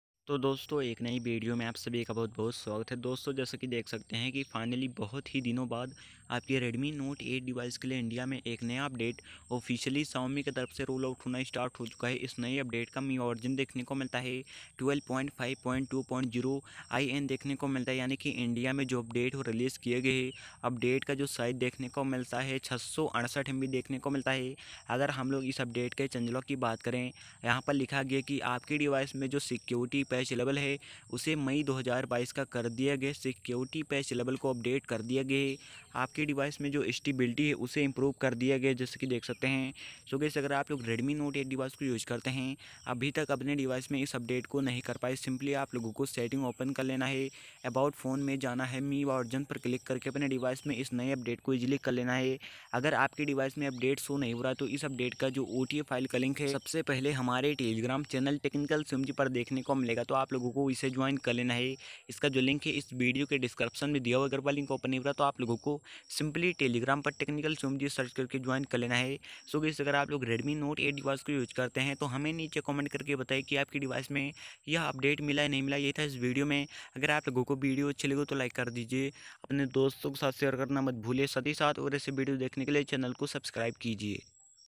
Redmi Note 8 MIUI 12.5.2.0 India New Update Rollout | Redmi Note 8 New Update OTA Link
Jai Mahadev | Jai Mahakaal

Search On Telegram 👉 Technical Shivam Ji